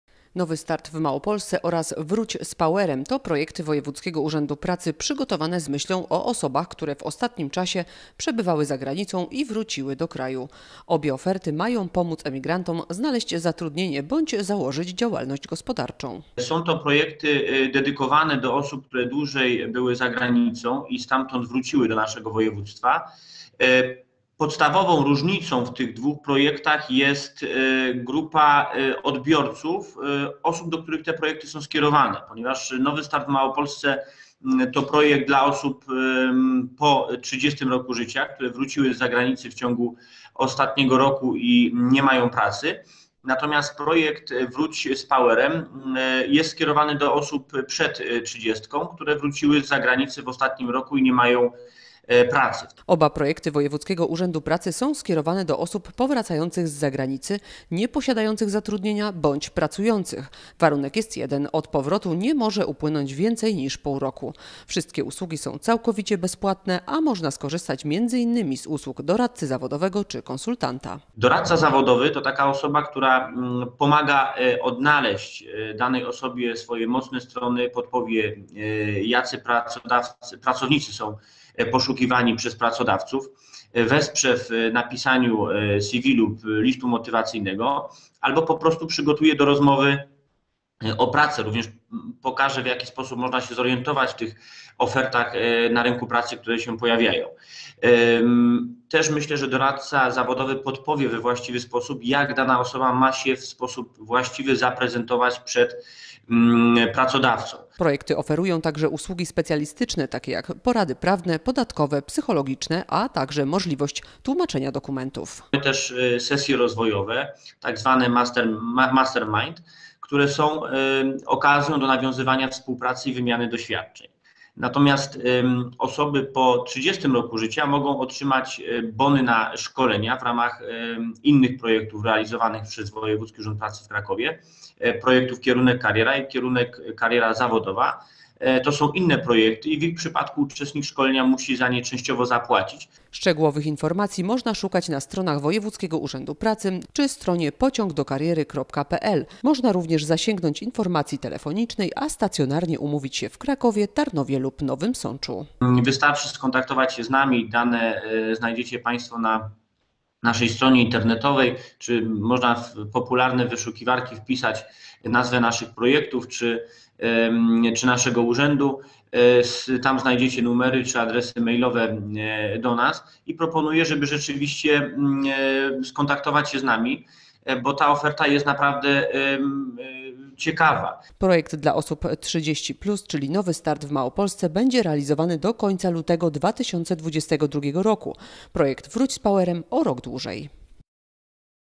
Wróciłeś z zagranicy? Skorzystaj z bezpłatnego wsparcia
Nowy start w Małopolsce i Wróć z Powerem to projekty realizowane przez WUP w Krakowie z myślą o osobach powracających z zagranicy. W ramach oferty można skorzystać z doradztwa zawodowego, porad prawnych, podatkowych i tłumaczeń.